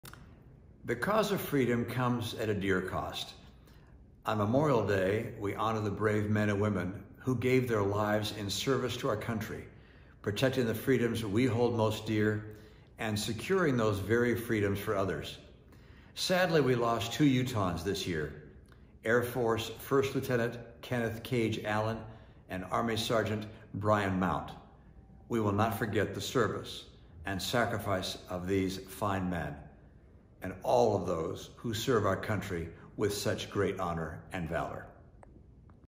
Honoring the men and women who have given their lives in service to our country.
The cause of freedom comes at a dear cost. On Memorial Day, we honor the brave men and women who have given their lives in service to our country. Sadly, we lost two Utahns this past year—Air Force 1st Lieutenant Kenneth “Kage” Allen and Army Sergeant Bryan Mount. We will never forget the service and sacrifice of these fine men, and all of those who serve our country with such great honor.